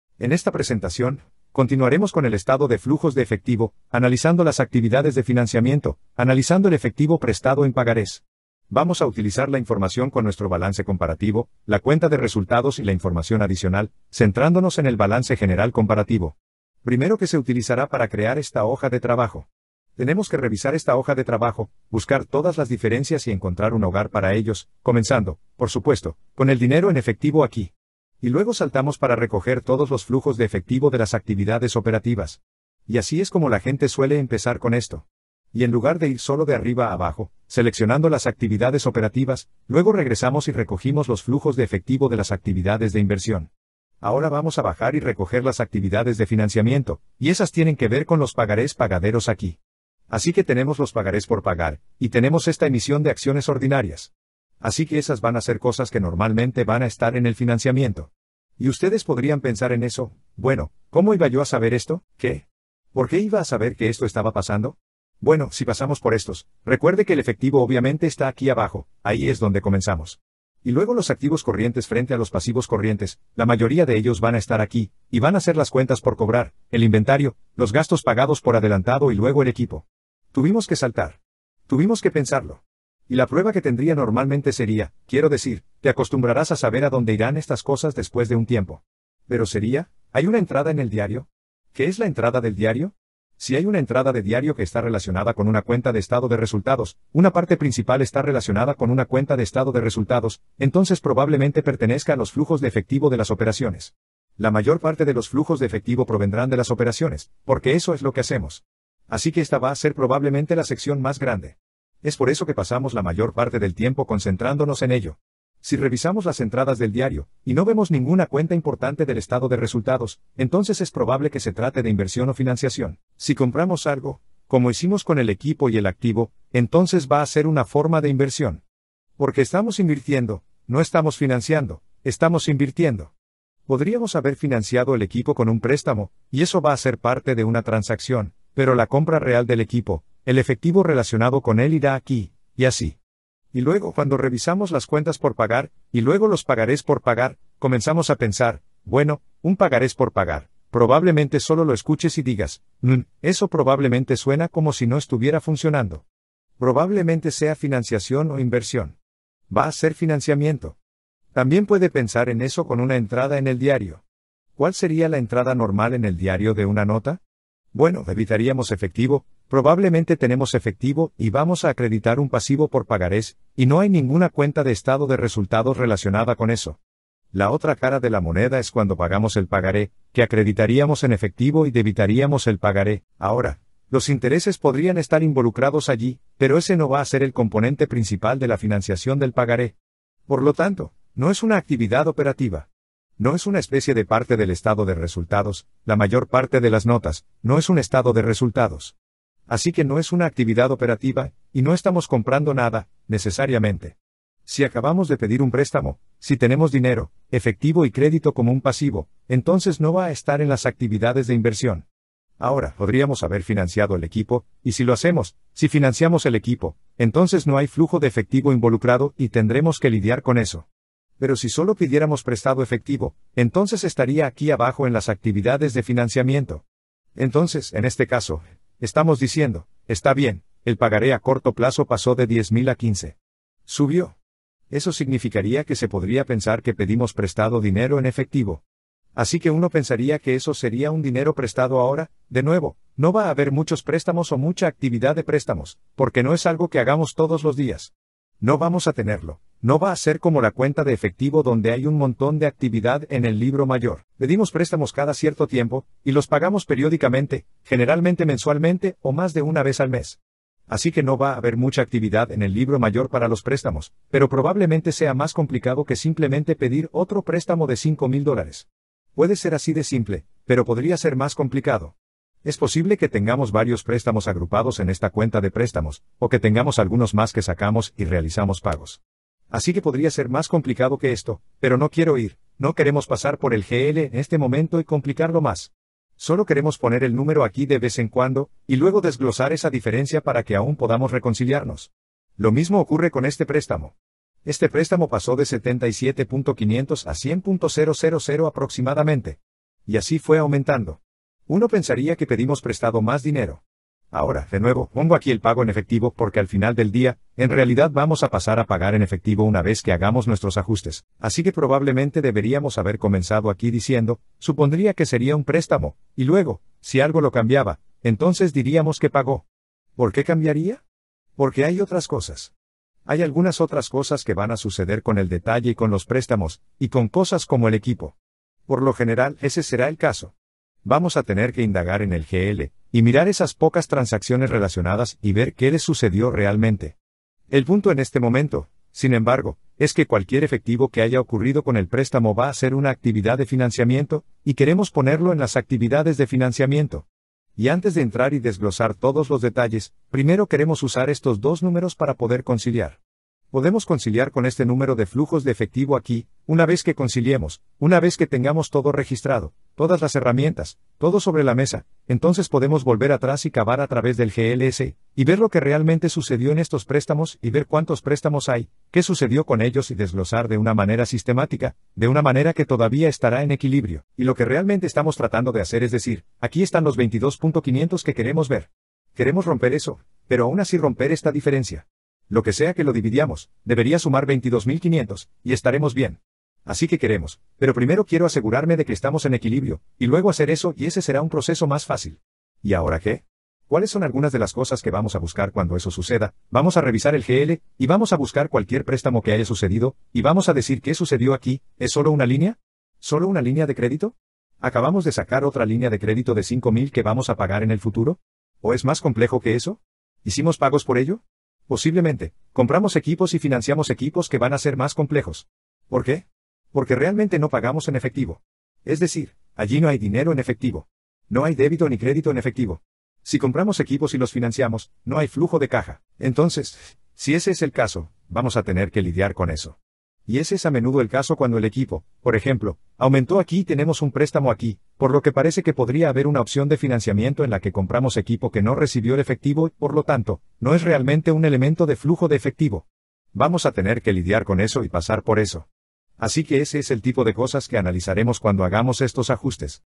Estado de flujo de efectivo Actividades de financiación Efectivo prestado en pagarés 160
Estado de flujo de efectivo Actividades de financiación Efectivo prestado en pagarés
Contabilidad financiera